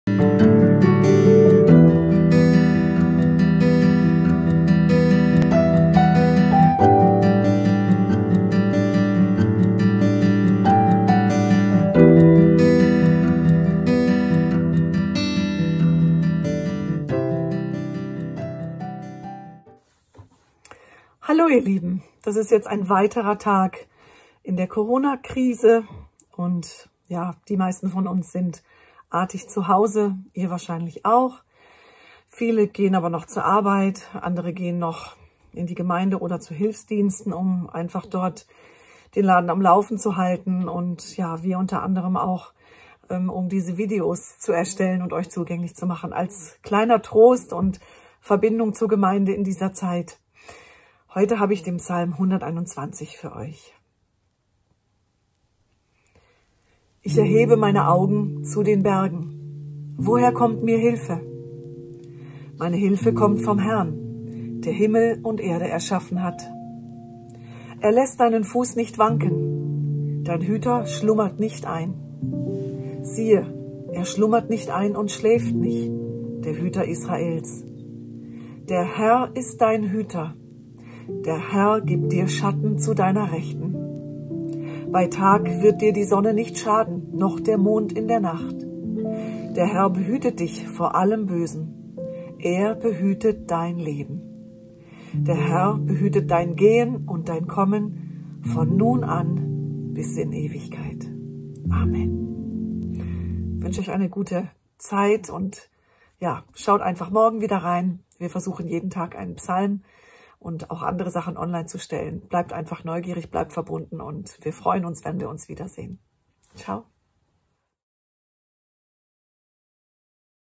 Lesung des Wortes vom 30.März - Psalm 121
Psalm 121 aus der Bibel gelesen von Katinka Ackermann